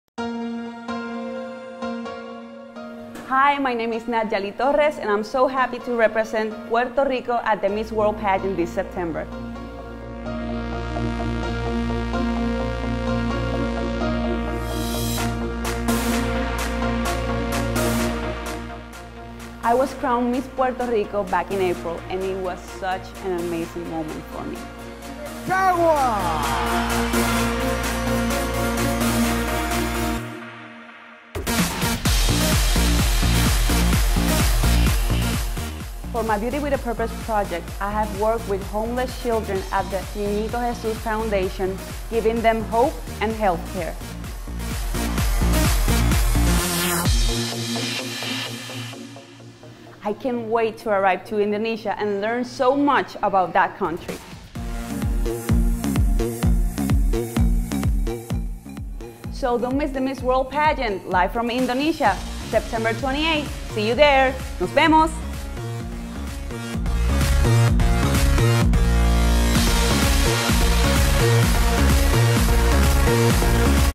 Miss World 2013 - Puerto Rico - Contestant Introduction
An introduction to Nadyalee Torres, Puerto Rico's entry to Miss World 2013!